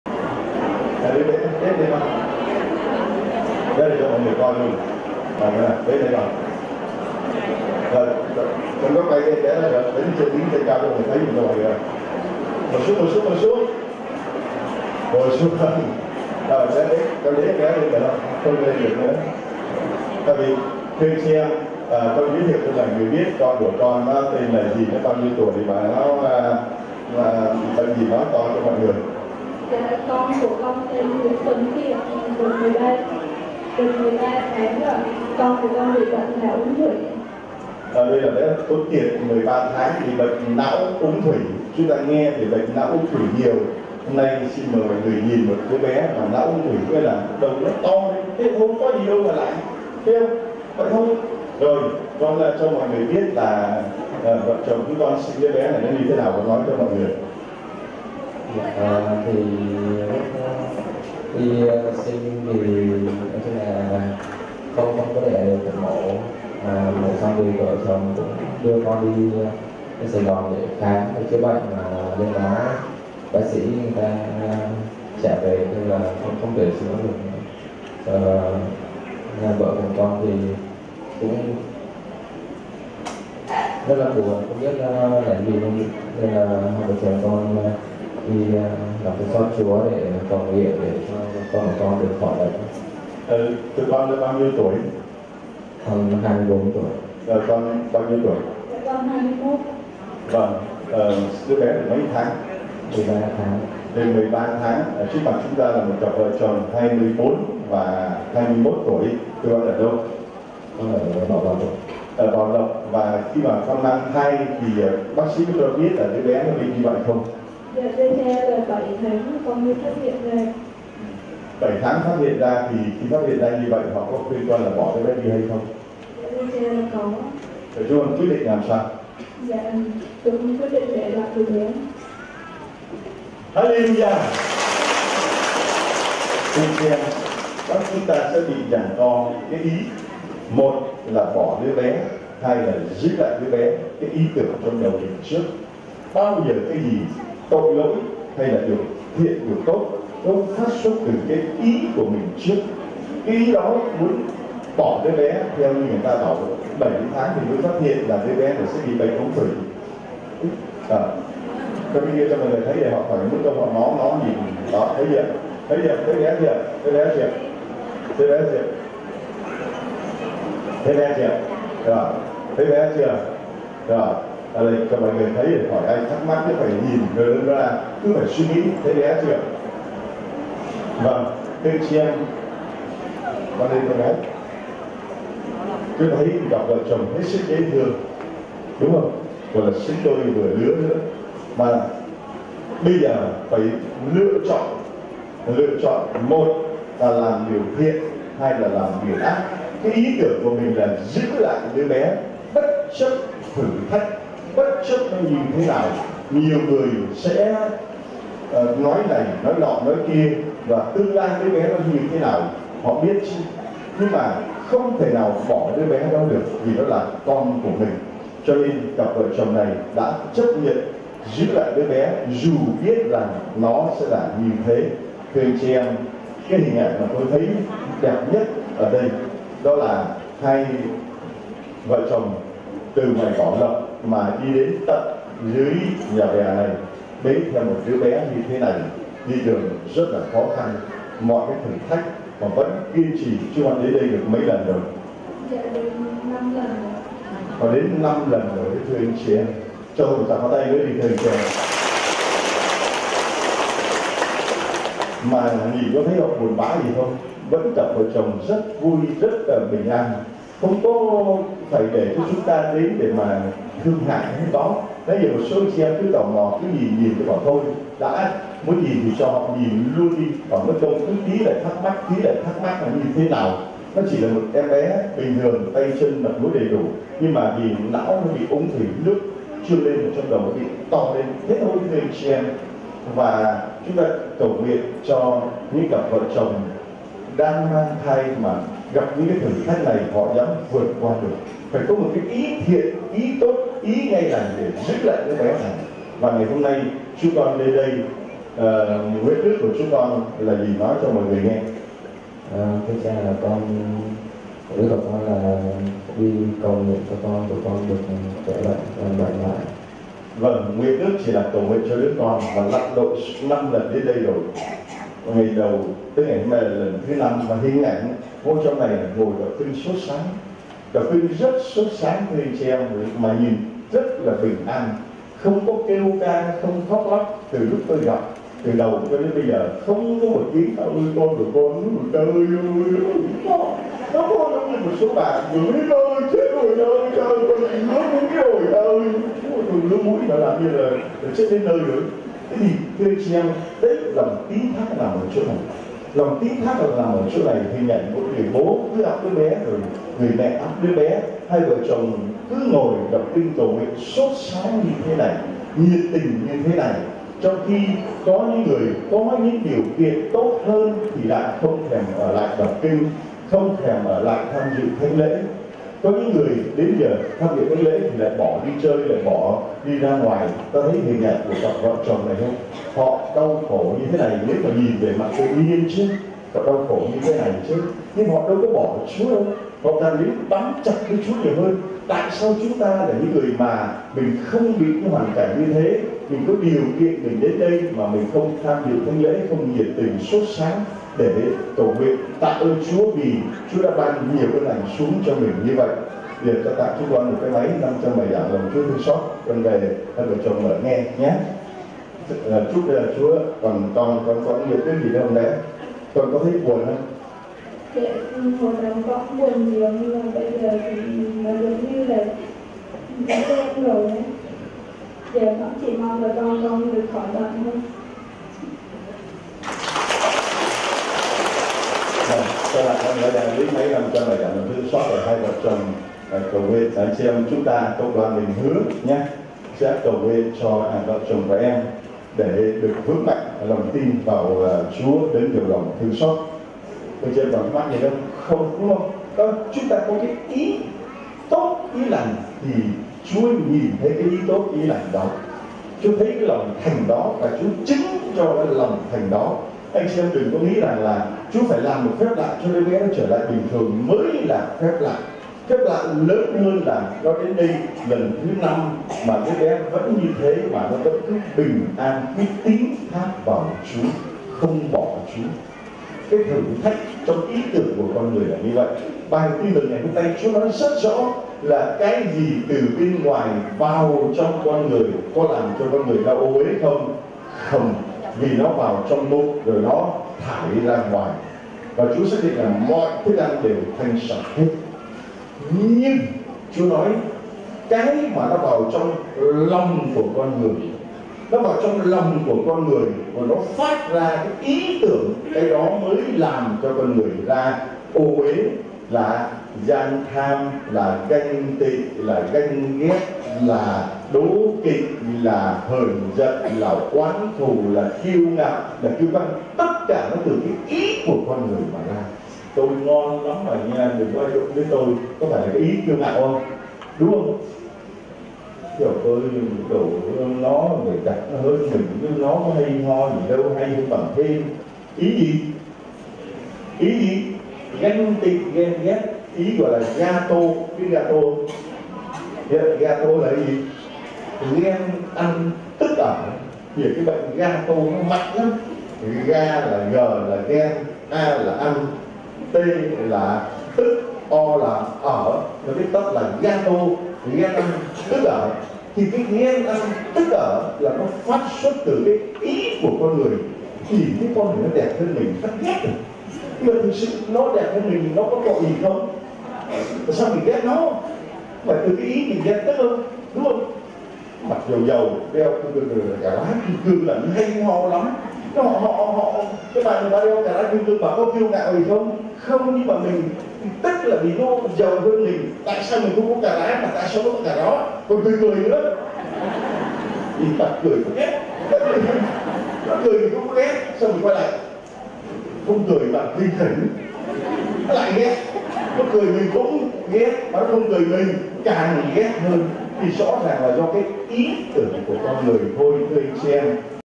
CHA LONG- LÒNG THƯƠNG XÓT CHÚA- Bé bị não úng thủy, bố mẹ trẻ vững tin tuyệt đối vào LCTX
2 VC chồng trẻ phát hiện thai nhị bị não úng thủy lúc 7 tháng tuổi, bác sĩ bảo bỏ, nhưng vợ chồng tín thác LCTX và cương quyết giữ bé.

Cha Long giảng giải về việc phải lựa chọn giữ thiện và ác !

Lạy Chúa Giêsu, con tín thác vào Chúa !